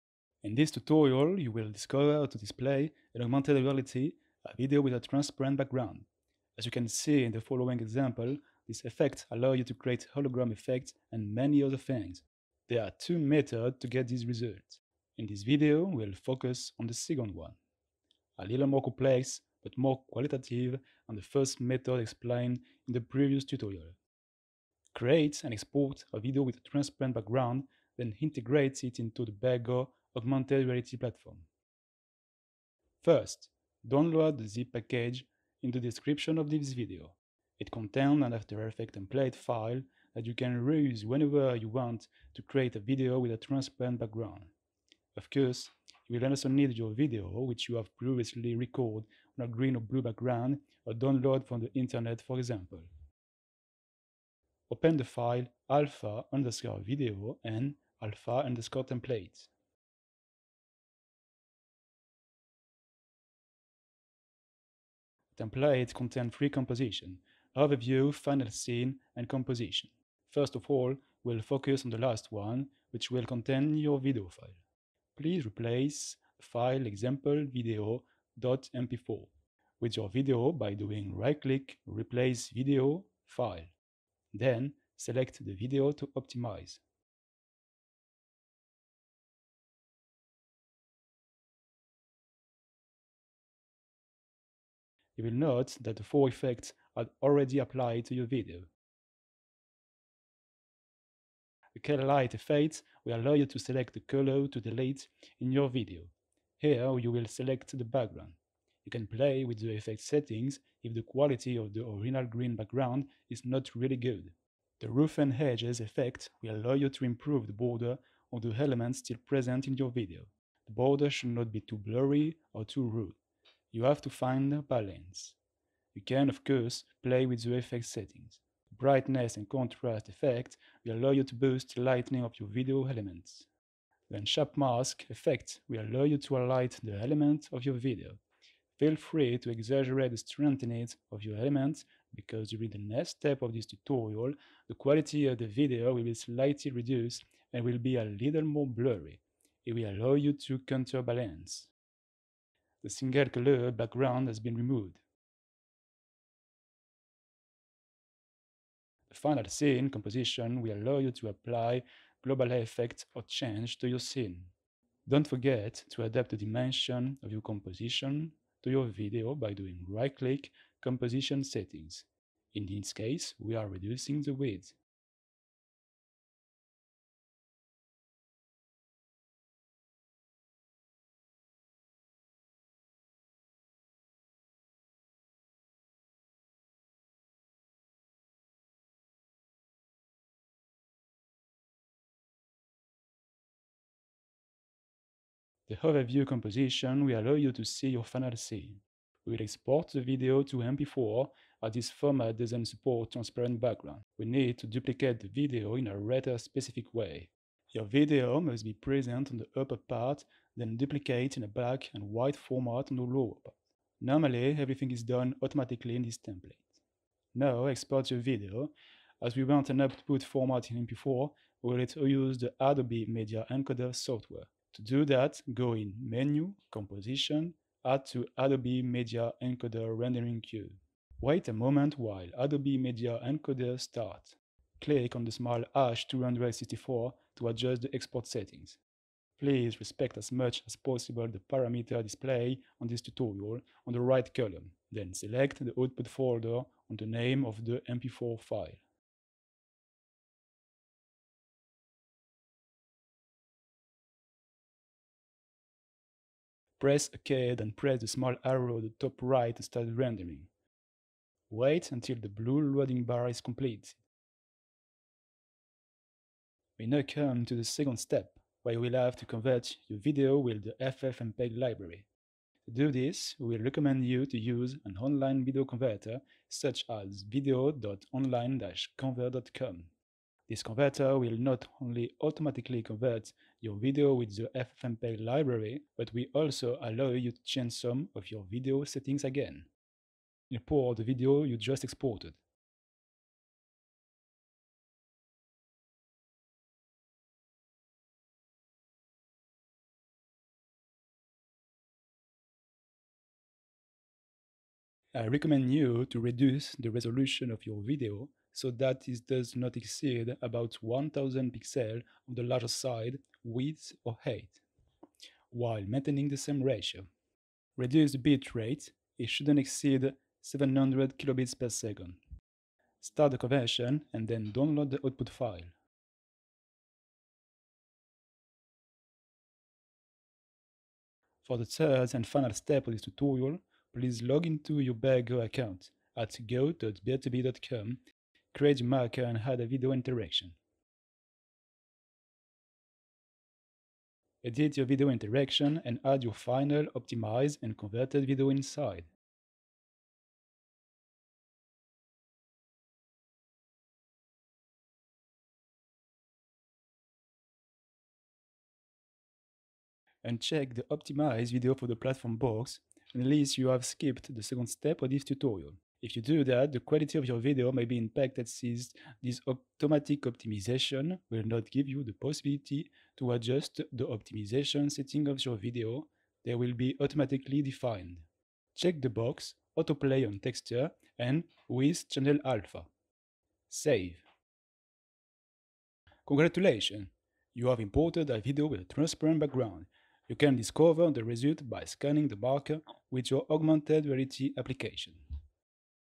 Display video with transparent background in AR - Method 2
Discover how to display, in augmented reality, a video with a transparent background. The second method explained in this tutorial will use videos with alpha channel.

Part 1: 00:33 - 04:25 : Create/Optimize your video from After Effects
Part 2: 04:26 - 05:31 : Convert your video online
Part 3: 05:32 - 06:36 : Import your video in the BearGO augmented reality platform

- Your video (with a channel alpha or a unicolor background)
- After Effects, Premiere Pro or other video editing software
- An account in the BearGO augmented reality platform, linked to your application.